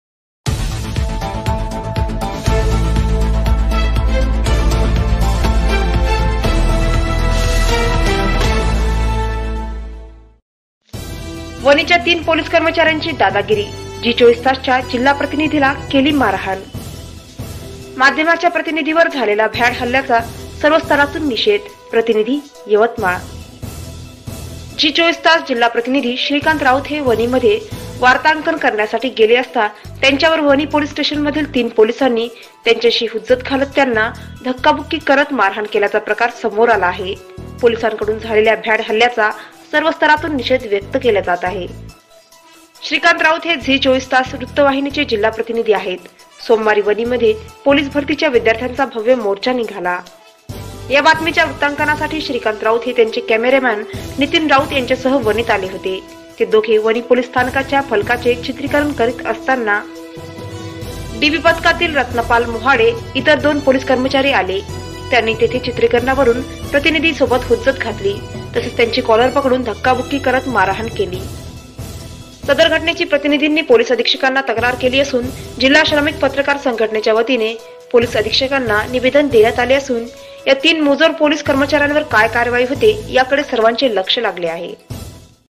वणीच्या तीन पोलीस कर्मचाऱ्यांची "दादागिरी"...झी 24 तासच्या जिल्हा प्रतिनिधी ला केली मारहाण...
झी 24 तासच्या जिल्हा प्रतिनिधी ला केली मारहाण.

माध्यमाच्या प्रतिनिधी वर झालेला भ्याड हल्ल्याचा सर्व स्तरातून निषेध!

प्रतीनिधी/यवतमाळ

झी 24 तास जिल्हा प्रतिनिधी श्रीकांत राऊत हे वणीमध्ये  वार्तांकन करण्यासाठी गेले असता त्यांच्यावर वणी पोलीस स्टेशन मधील तीन पोलिसांनी त्यांच्याशी हुज्जत घालत त्यांना धक्काबुक्की करत मारहाण केल्याचा प्रकार समोर आला आहे. पोलिसाकडून झालेल्या भ्याड हल्ल्याचा सर्व स्तरातून निषेध व्यक्त केल्या जात आहे.

श्रीकांत राऊत हे झी 24 तास या वृत्तवाहिणीचे जिल्हा प्रतिनिधी आहेत. सोमवारी वणीमध्ये पोलीस भरतीच्या विद्यार्थ्यांचा भव्य मोर्चा निघाला. या बातमीच्या वृत्तांकनासाठी श्रीकांत राऊत हे त्यांच्या कॅमेरामन नितीन राऊत यांच्यासह वणीत आले होते. ते दोघे वणी पोलीस स्थानकाच्या फलकाचे चित्रिकरण करीत असताना डीबी पथकातील रत्नपाल मोहाडे ईतर दोन पोलीस कर्मचारी आले. त्यांनी तिथे चित्रीकरणावरून प्रतिनिधीं सोबत हुज्जत घातली तसेच त्यांची कॉलर पकडून धक्काबुक्की करत मारहाण केली.

सदर घटनेची प्रतिनिधीने पोलीस अधीक्षकांना तक्रार केली असून जिल्हा श्रमिक पत्रकार संघटनेच्या वतीने पोलीस अधीक्षकांना निवेदन देण्यात आले असून या तीन मुजोर पोलीस कर्मचाऱ्यांवर काय कारवाई होते याकडे सर्वांचे लक्ष लागले आहे.